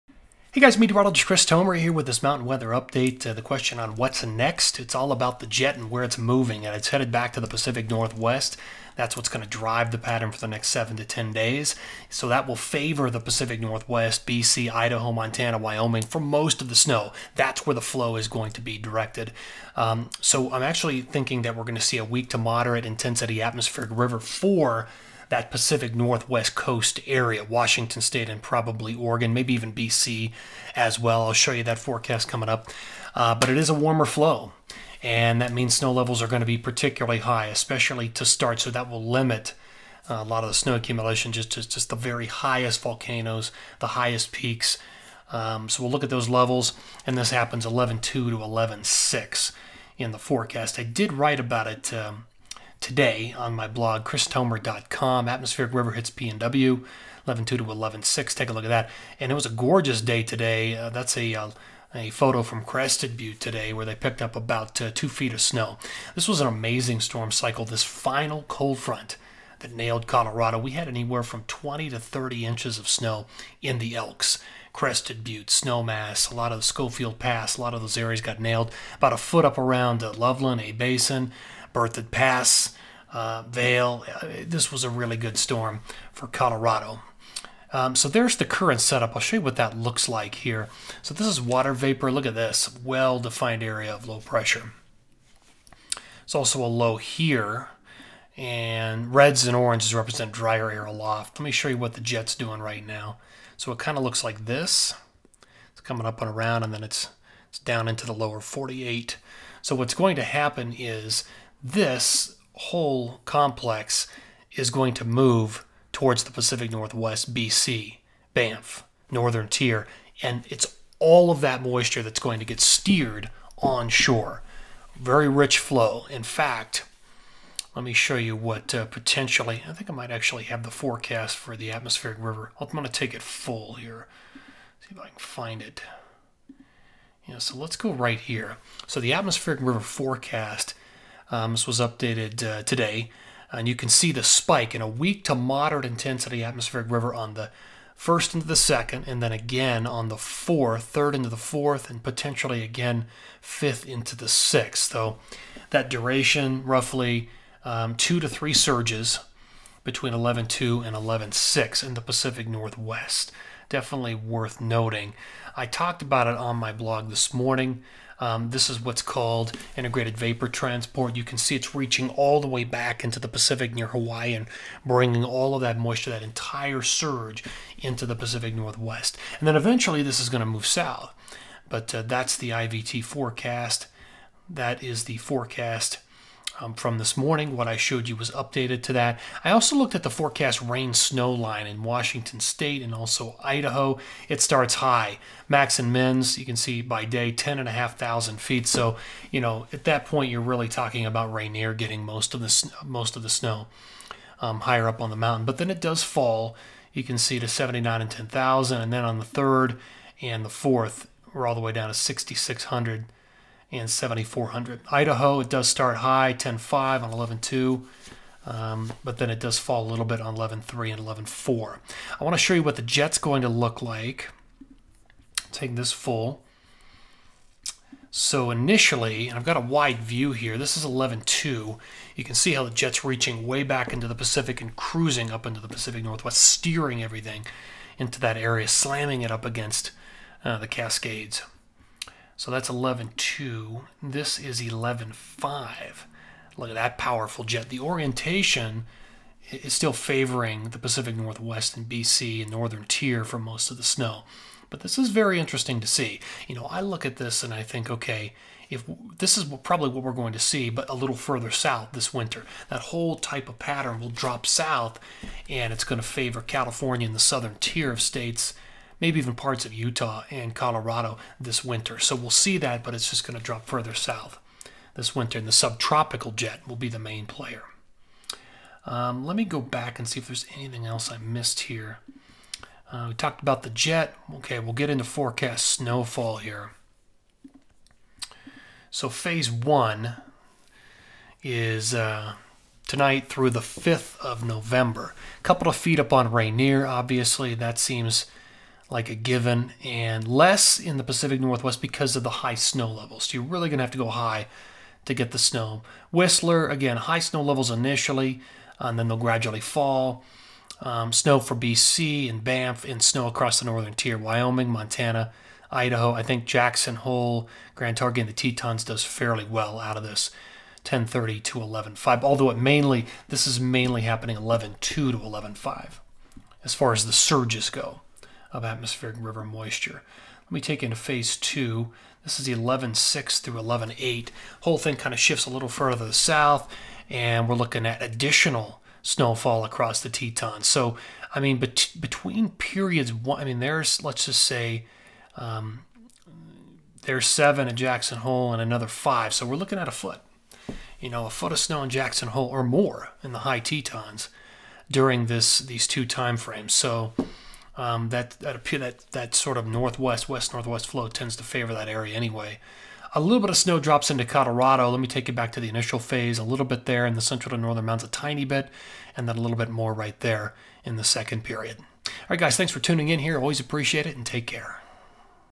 Mountain Weather update 10/30, Meteorologist Chris Tomer
What's next?  Atmospheric River (AR) of weak to moderate intensity for PNW/BC and northern Tier 11/2-11/6.